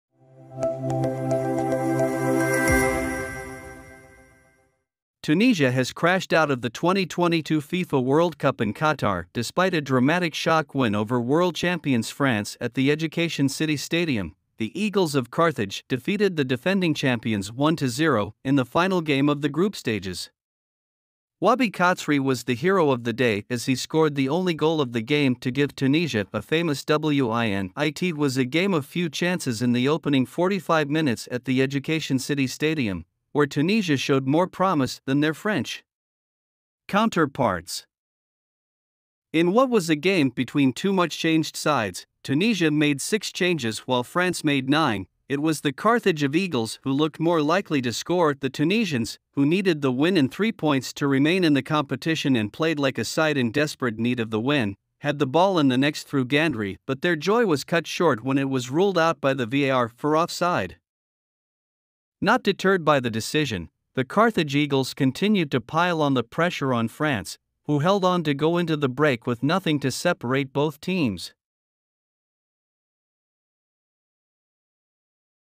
Tunisia out of World Cup despite dramatic win over much-changed France
Tunisia has crashed out of the 2022 FIFA World Cup in Qatar despite a dramatic shock win over World Champions France at the Education City Stadium.The Eagles of Carthage defeated the defending champions 1-0 in the final game of the group stages.Wahbi Khazri was the hero of the day as he scored the only goal of the game to give Tunisia a famous win.It was a game of few chances in the opening 45 minutes at the Education City Stadium, where Tunisia showed more promise than their French counterparts.

In what was a game between two much-changed sides, Tunisia made six changes while France made nine, it was the Carthage of Eagles who looked more likely to score.The Tunisians, who needed the win and three points to remain in the competition and played like a side in desperate need of the win, had the ball in the next through Ghandri but their joy was cut short when it was ruled out by the VAR for offside.

Not deterred by the decision, the Carthage Eagles continued to pile on the pressure on France, who held on to go into the break with nothing to separate both teams.